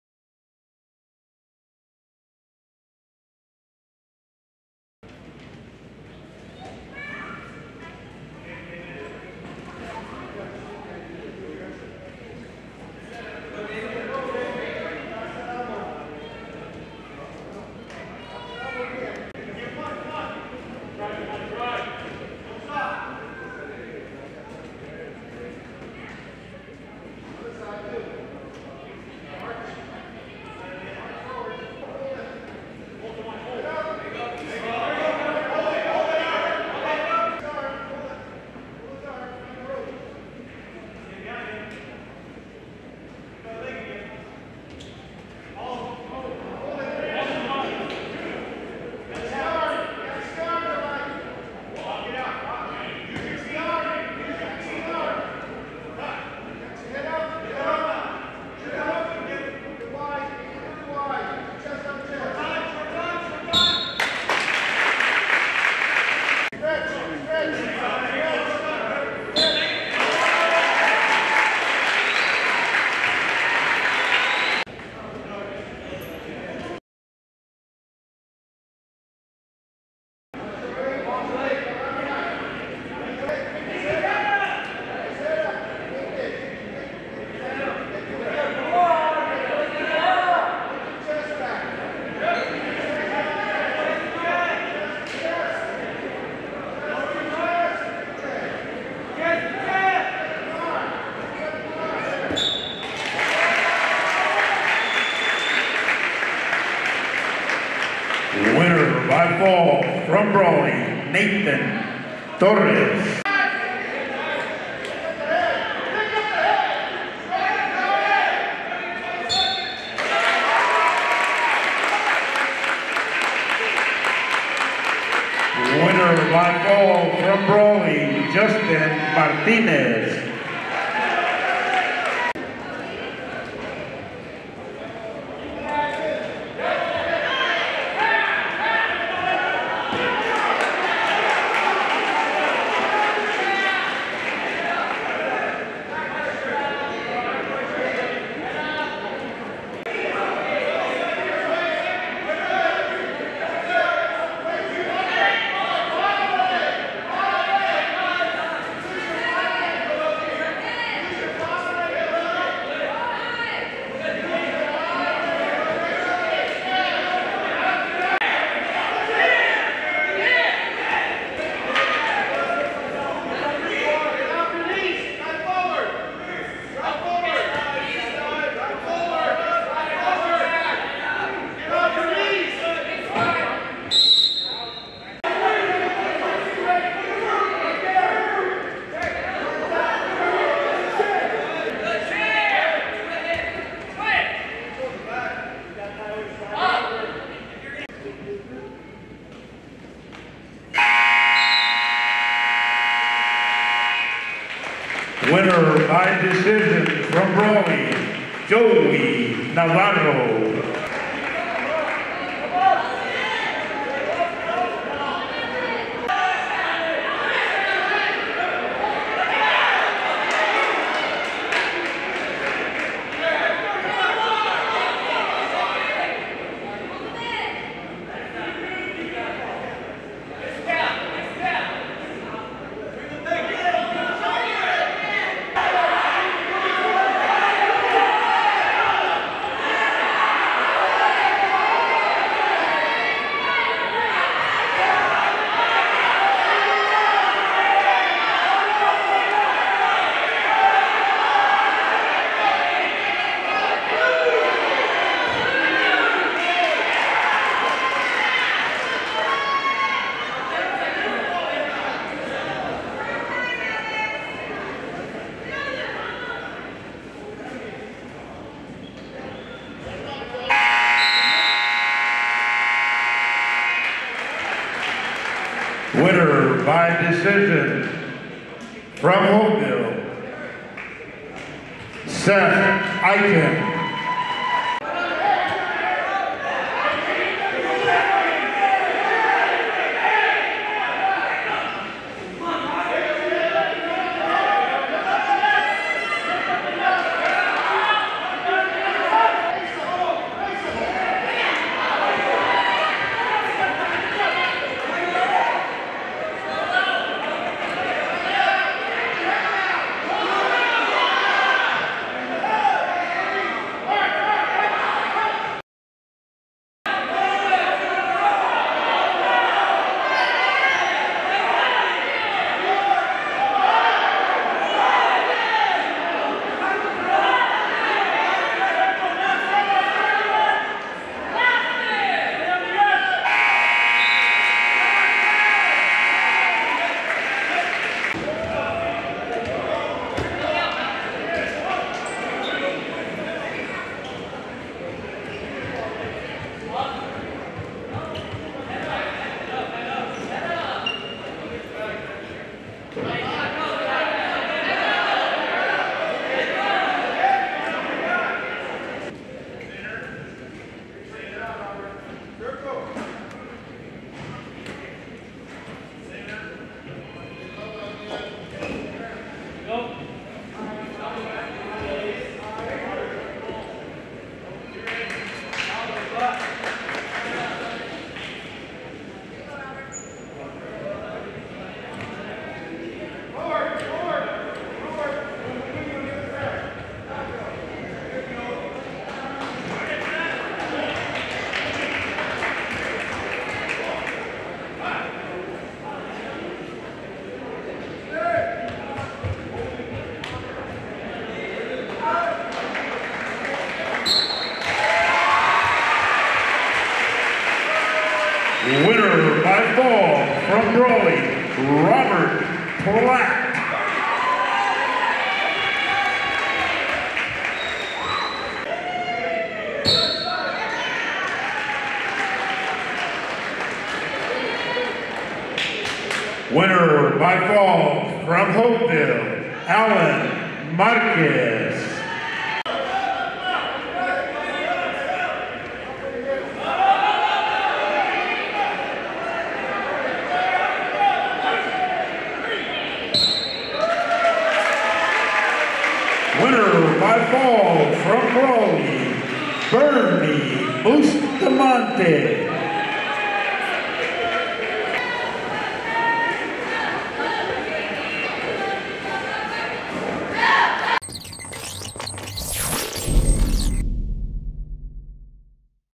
Boys Wrestling Tri-Meet Highlights: Palo Verde/Holtville/Brawley (1/26/2022)
Highlights from the Imperial Valley League boys wrestling tri-meet between Palo Verde, Holtville, and Brawley on January, 26, 2022 at Brawley Union High School in Brawley, CA.